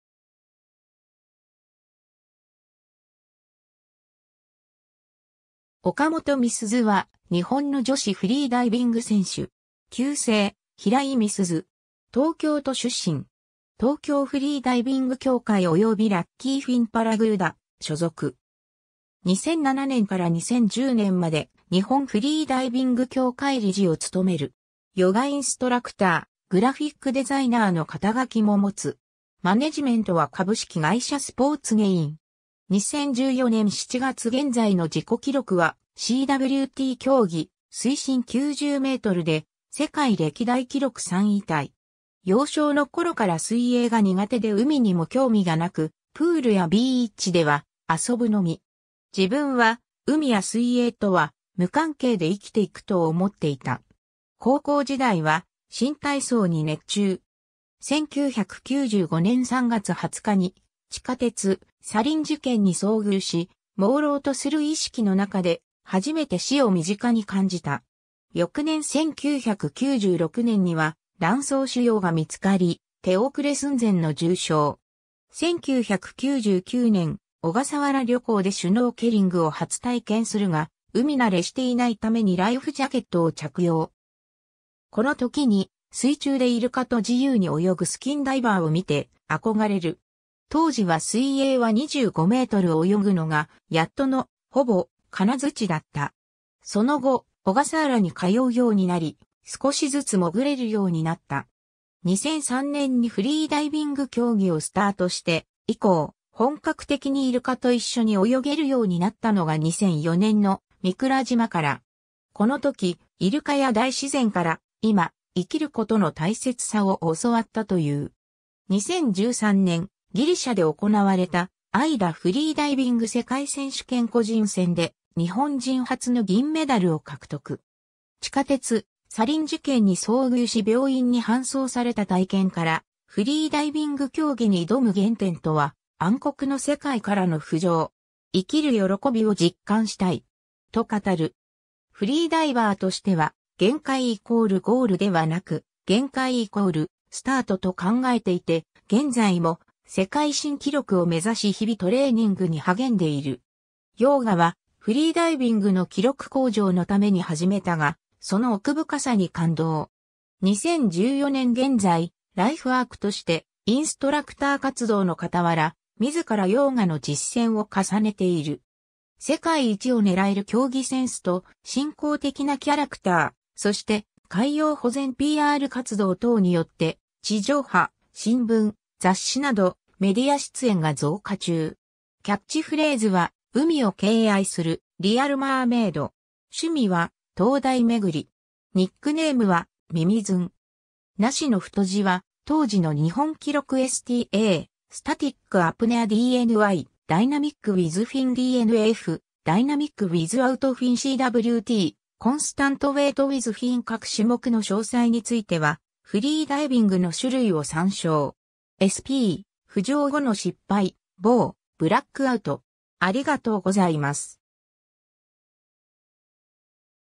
岡本美鈴
岡本 美鈴（おかもと みすず、1973年1月30日 - ）は、日本の女子フリーダイビング選手。
旧姓、平井 美鈴（ひらい みすず）。
東京都出身。
 東京フリーダイビング協会(TFC) およびラッキーフィンパラグーダ所属。
2007年から2010年まで、日本フリーダイビング協会(JAS)理事を務める。
ヨガインストラクター（日本ヨーガ学会）、グラフィックデザイナーの肩書も持つ。
マネジメントは株式会社スポーツゲイン。
2014年7月現在の自己記録は、CWT競技・水深90mで、世界歴代記録3位タイ。
 幼少の頃から水泳が苦手で海にも興味がなく、プールやビーチでは遊ぶのみ。
自分は海や水泳とは無関係で生きていくと思っていた。
高校時代は新体操に熱中。
 1995年3月20日に地下鉄サリン事件に遭遇し、もうろうとする意識の中で初めて死を身近に感じた。
翌年1996年には卵巣腫瘍が見つかり、手遅れ寸前の重傷。
 1999年、小笠原旅行でシュノーケリングを初体験するが、海慣れしていないためにライフジャケットを着用。
この時に水中でイルカと自由に泳ぐスキンダイバーを見て憧れる。
当時は水泳は25mを泳ぐのがやっとの、ほぼ「カナヅチ」だった。
その後、小笠原に通うようになり、少しずつ潜れるようになった。
2003年にフリーダイビング競技をスタートして以降、本格的にイルカと一緒に泳げるようになったのが2004年の御蔵島から。
この時、イルカや大自然から「今、生きる事の大切さ」を教わったと言う。
 2013年、ギリシャで行われたAIDAフリーダイビング世界選手権個人戦で、日本人初の銀メダルを獲得。
地下鉄サリン事件に遭遇し病院に搬送された体験から、フリーダイビング競技に挑む原点とは「暗黒の世界からの浮上。生きる喜びを実感したい。」と語る。
 フリーダイバーとしては、「限界＝ゴール」ではなく、「限界＝スタート」と考えていて、現在も世界新記録を目指し日々トレーニングに励んでいる。
 ヨーガはフリーダイビングの記録向上のために始めたが、その奥深さに感動。
2014年現在、ライフワークとしてインストラクター活動の傍ら、自らヨーガの実践を重ねている。
 世界一を狙える競技センスと親交的なキャラクター、そして海洋保全PR活動等によって、地上波・新聞・雑誌などメディア出演が増加中。
キャッチフレーズは「海を敬愛するリアル・マーメイド」。
 趣味は「灯台巡り」。
ニックネームは「みみずん（海外ではMimi）」。
 ※（ ）なしの太字は当時の日本記録 STA…スタティック・アプネア DYN…ダイナミック・ウィズ・フィン DNF・・・ダイナミック・ウィズアウト・フィン CWT・・・コンスタント・ウェイト・ウィズ・フィン 各種目の詳細についてはフリーダイビングの種類を参照。
 SP…浮上後の失敗（失格） BO…ブラックアウト（失格）